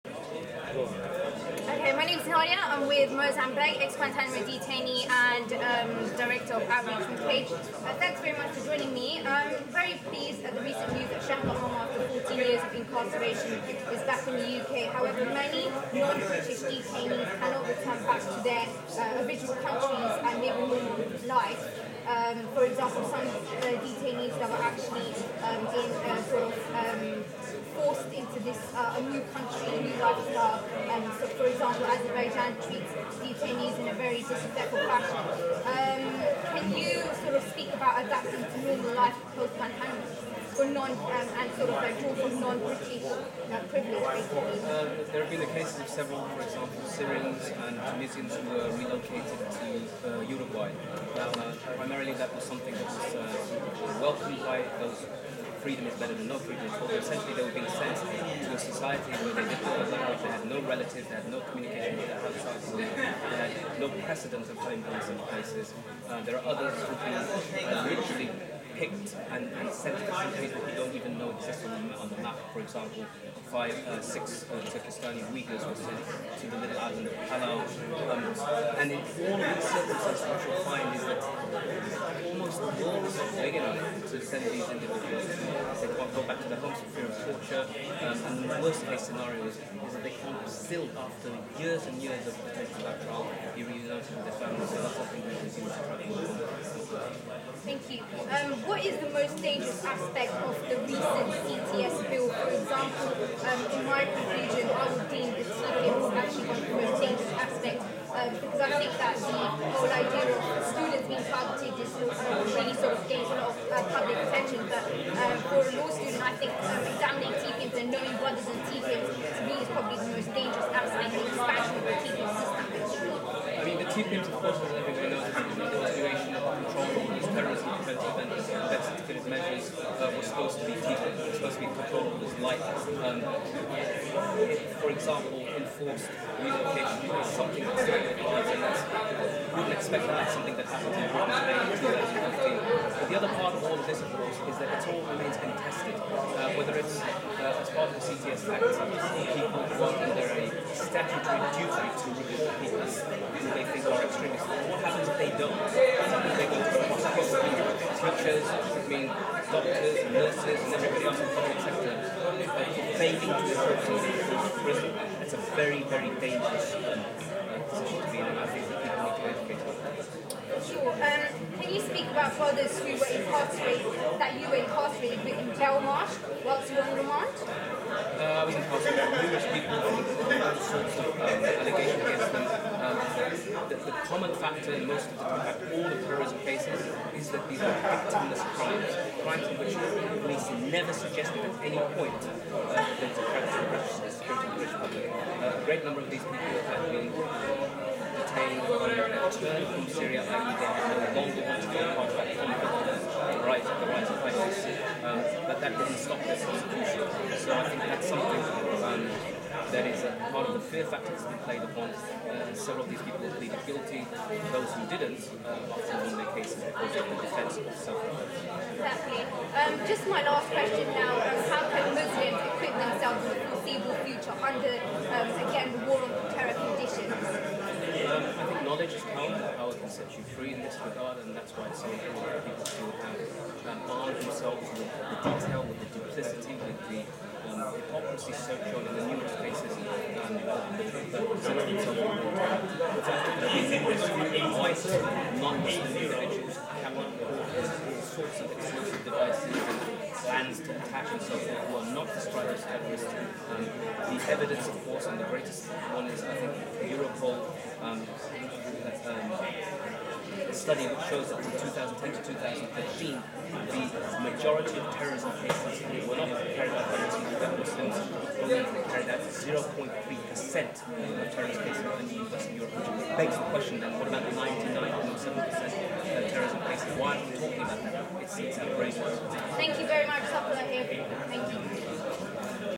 Brothers behind bars: Nadia speaks to Moazzam Begg ex Guantanamo Prisoner
Transcript of the conversation with Moazzam Begg, former detainee at Guantanamo and Outreach director for CAGE.
Q) Very pleased at the recent news that Shaker Aamer after 14 years of incarceration is back in the UK - however many non British Muslim detainees cannot return to their country of origin or live a normal life after Guantanamo. For instance detainees are treated very badly in Azerbaijan. Can you speak about adopting to a normal lifestyle post Guantanamo?
Moazzam: there have cases, for example of several Syrians and Tunisians who were relocated to Uruguay, they were sent to a society where they didn't know the language, no relatives and no communication. There have been others who have been literally sent to places that you didn't know even existed on the map - for example Turkistani Uyghur Muslims were sent to Guantanamo, for some of them if they were repatriated to their homelands some would be subject to more torture, this is the worse case scenario and so after years and years of detention without trial cannot be reunited with their families.
Q) What is the most dangerous aspect of the Counter Terrorism and Security [CTS] Bill? [from my conclusion it the expansion of the TPIM system - Terrorism Prevention and Investigation Measures ], in the public there has been a lot of attention on targeting students [which is very alarming of course] but not enough education on the TPIM measures
Moazzam: TPIMS are re-branding the control order regimes that were scrapped for being too draconian. TPIMS can lead to enforced relocation within Britain from one city to another, [this can entail uprooting families if an individual from this family is placed on a TPIM]. The other part of the CTS Bill of course is that people are now under a statutory duty to essentially report on fellow citizens within society - and what happens if they don't? Doctors, nurses, teachers and others in the public sector will 'report' on others which is a very dangerous and on what basis are they 'reporting people? We need to be highly critical about this.
Q) Can you speak about brothers you were incarcerated with inside Belmarsh whilst you were on remand for up to 7 months? [Moazzam was subsequently cleared of all charges against him]
Moazzam: There were numerous people from different backgrounds with all sorts of allegations against them, the common factor was that they were victims of crimes themselves, crimes which the British intelligence and the state have perpetuated, also a great number of people have been detained from their return from Syria and they no longer want to be part of the conflict due to the rise of ISIS but that didn't stop their prosecution, there is a fear factor that is played upon, some are found 'guilty' and there are many who have been cleared.
Q) What can Muslims do to equip themselves in the foreseeable future due to the 'war on terror' conditions?
Moazzam: Knowledge is power and power can set you free, this is why it's important for people to arm themselves with details and the duplicity & the hypocrisy of the British state. Many whites, and non Muslims are never described as 'terrorists' despite using all sorts of explosive devices and other means to attack civilians. Europol - a study from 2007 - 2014 shows that the majority of terrorist acts were not carried out by Muslims. Only 0.7% of terrorist cases were Muslims, which begs the question, what about the 99.3% of terrorism not committed by Muslims?